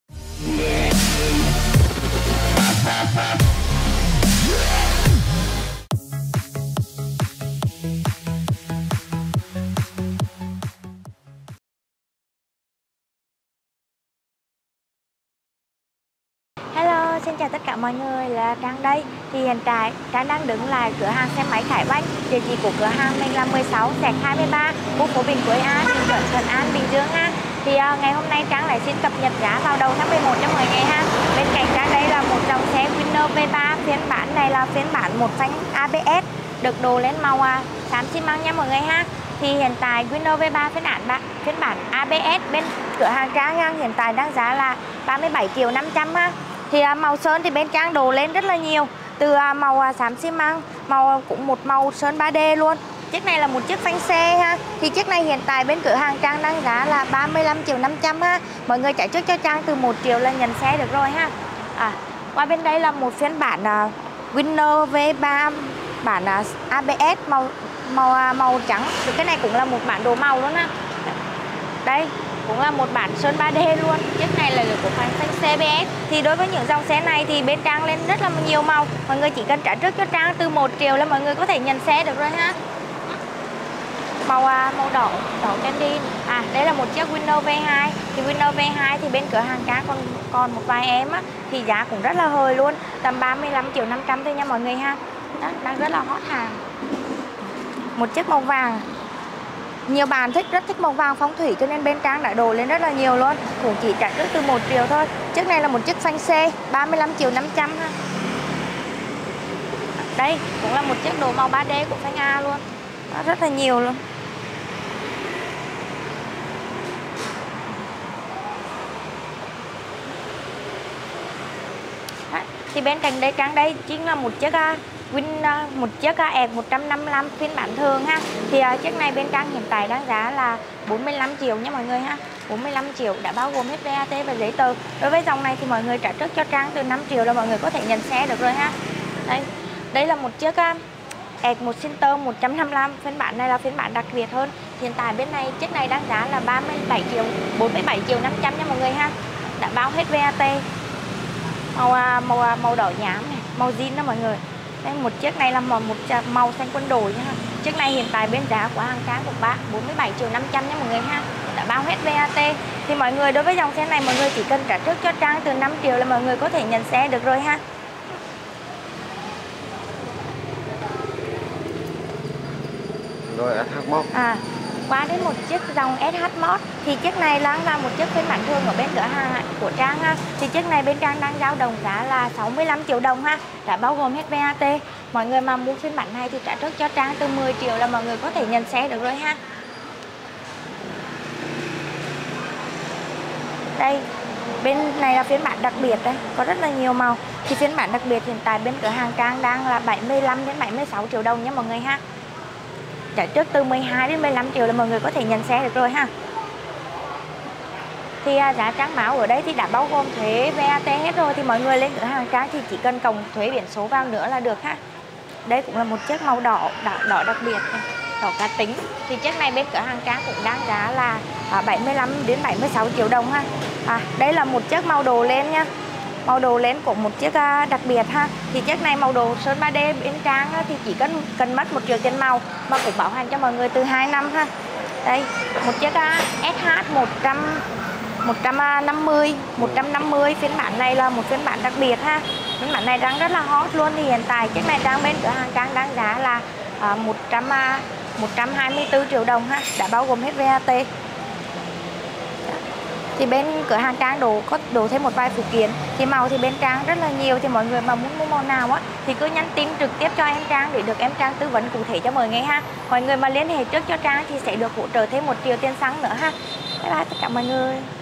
BẤT NGỜ GIÁ XE HONDA WINNER V3 EXCITER 155 SH MODE 2023 SH 150 RỚT GIÁ THẢM XE MÁY GIÁ RẺ
(y) Xe máy Khải Oanh Bình Dương
(y) Địa chỉ: 16/23, KP Bình Quới A, Phường Bình Chuẩn, TP Thuận An, Bình Dương